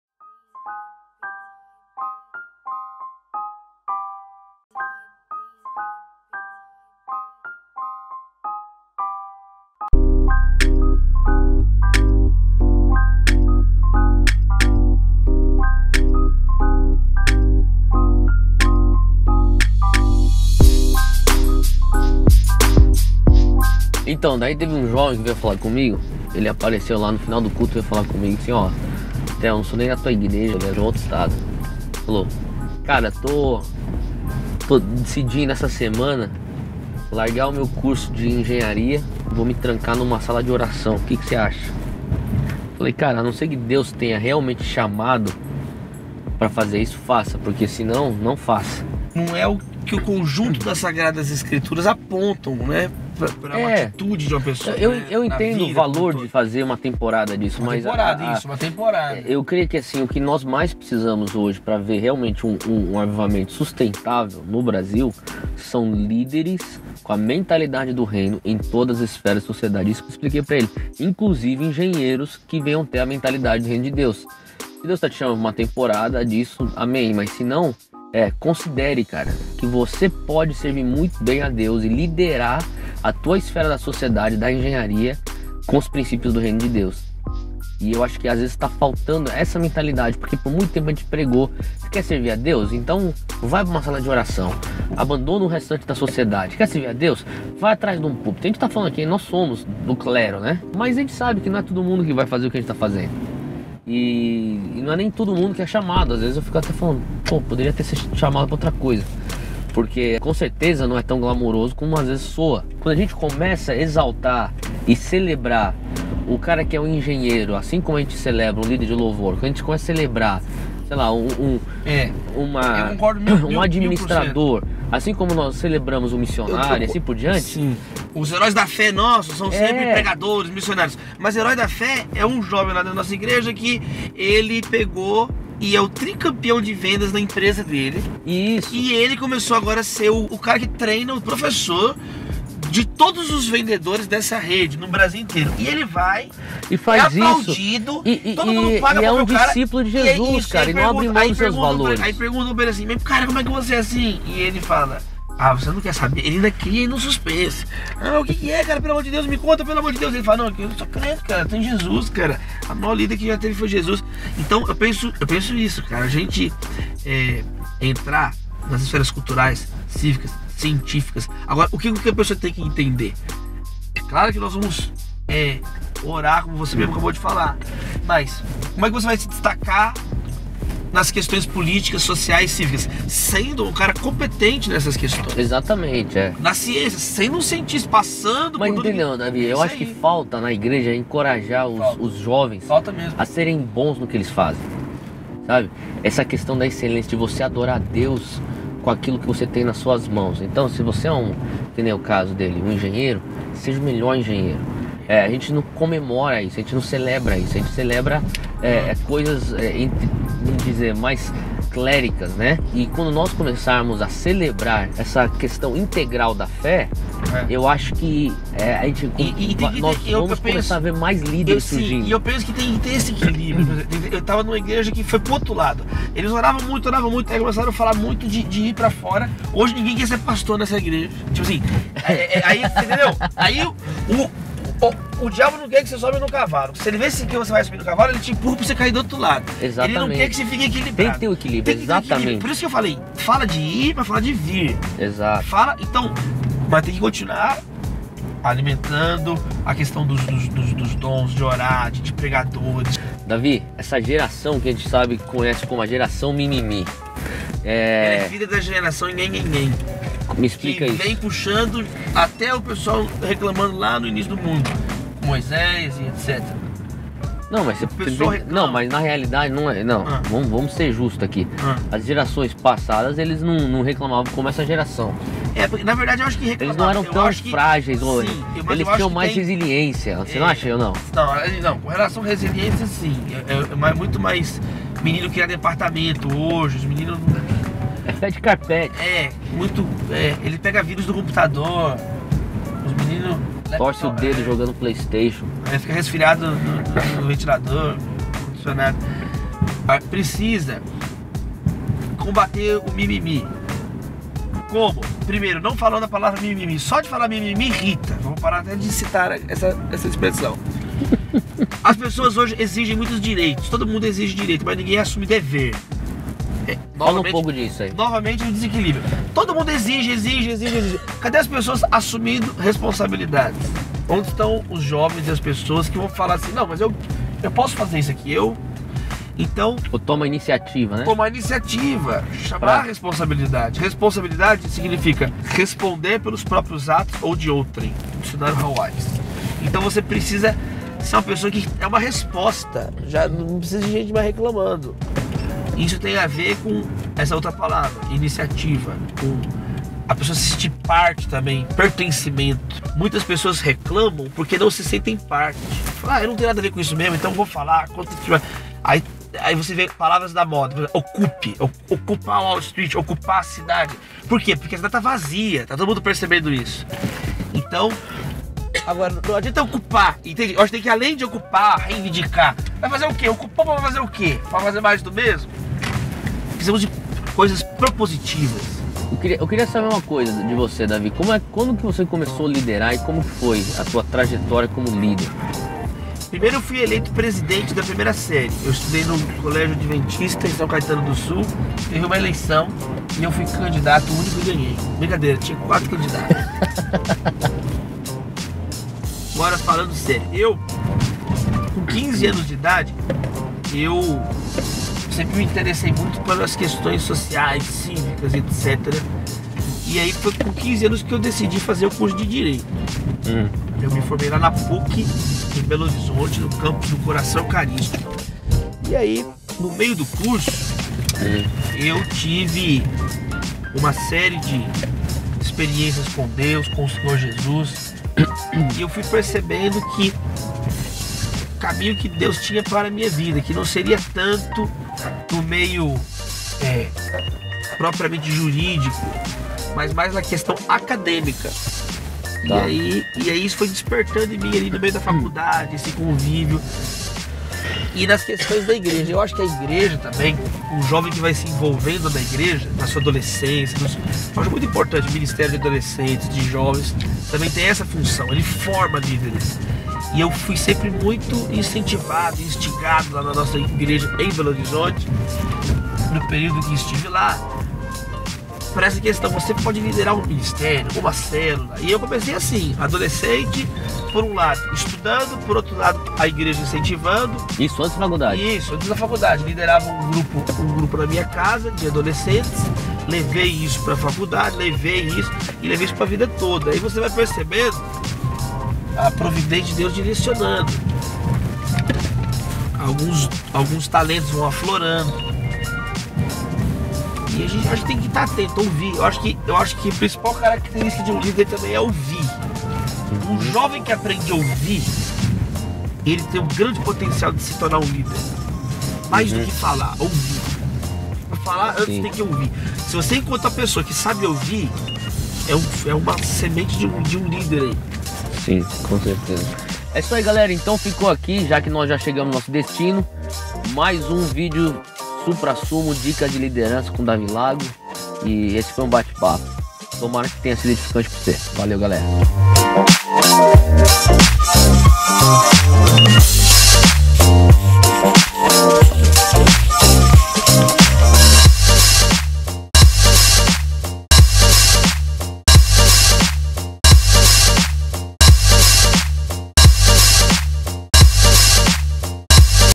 Supra Sumo #13 - Bate-papo com Davi Lago // Pt. 2
Se você busca ser uma resposta para a sociedade, confira esse novo episódio de SUPRA SUMO!

Na continuação deste bate-papo entre Teófilo Hayashi (@dunamisdude) e Davi Lago (@davilago), falamos sobre a necessidade de líderes com uma mentalidade do Reino em todas as esferas da sociedade e a importância de entender para o que cada um de nós foi chamado.

Aperte o PLAY e confira!